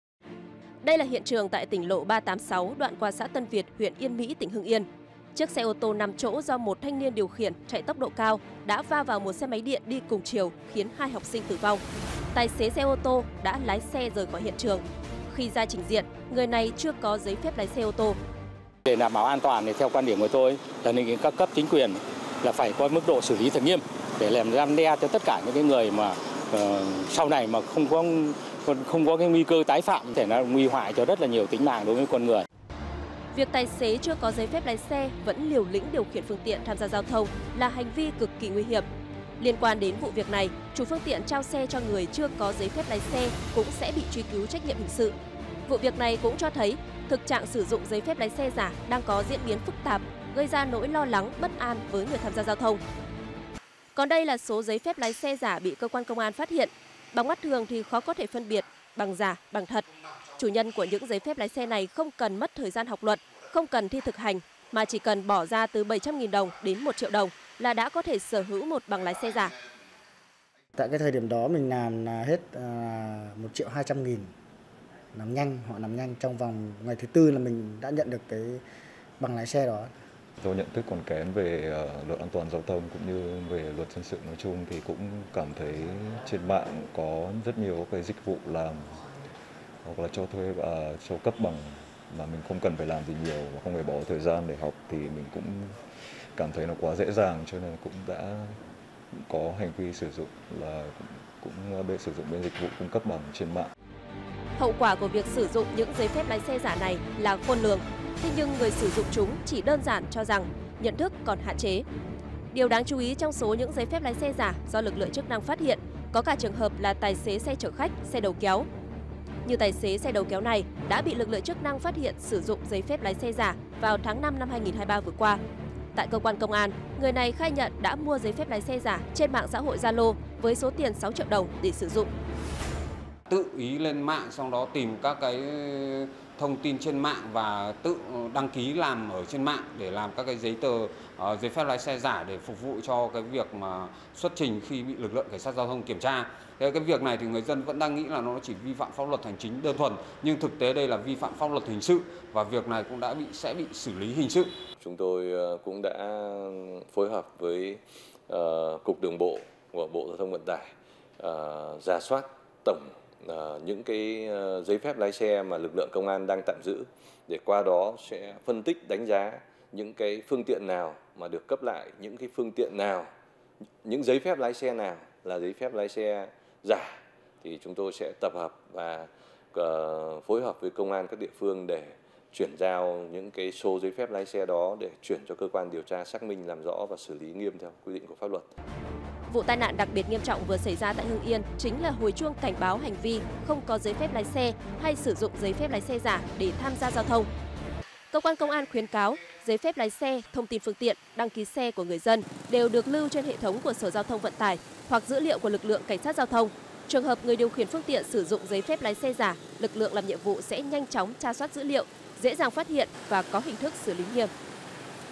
Không có giấy phép lái xe, sử dụng giấy phép giả và hệ luỵ | Tin nóng
Việc các lái xe chưa đủ tuổi không có GPLX hoặc sử dụng các loại giấy phép lái xe giả để tham gia giao thông trên đường thời gian gần đây có diễn biến phức tạp. Việc sử dụng GPLX giả không chỉ tiềm ẩn nguy cơ cao gây TNGT mà nó có dấu hiệu vi phạm pháp luật,  làm giả con dấu, chứng từ do cơ quan có thẩm quyền cấp…